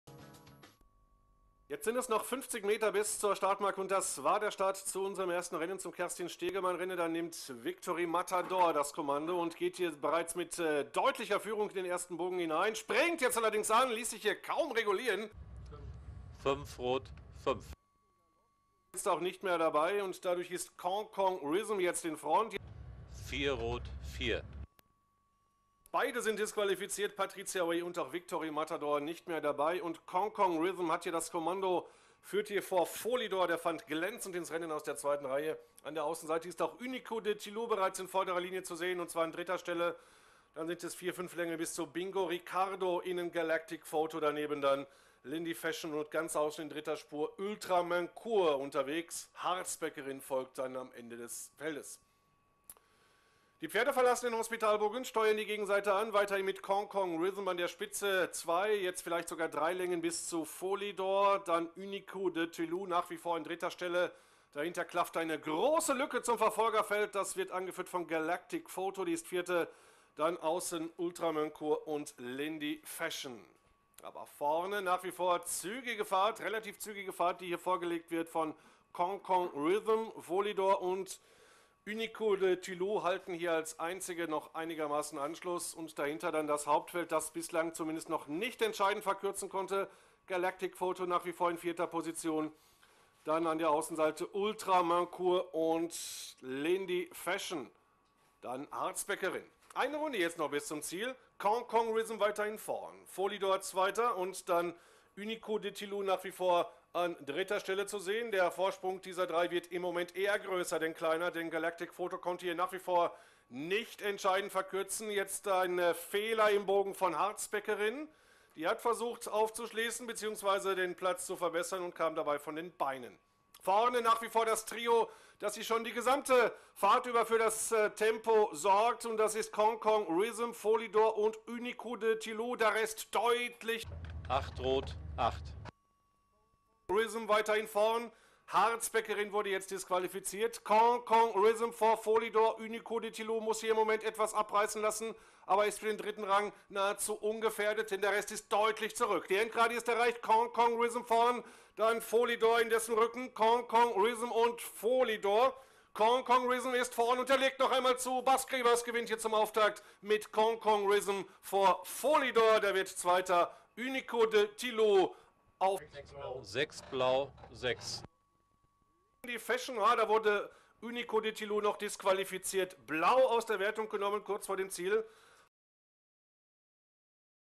Cancan Rhythm & Bas Crebas winnen Kerstin Stegemann-Rennen - Dinslaken 10 juni 2013
Cancan Rhythm & Bas Crebas (tr.) winnen Kerstin Stegemann-Rennen voor eig.combinatie Iwema/Terpstra - Dinslaken 10 juni 2013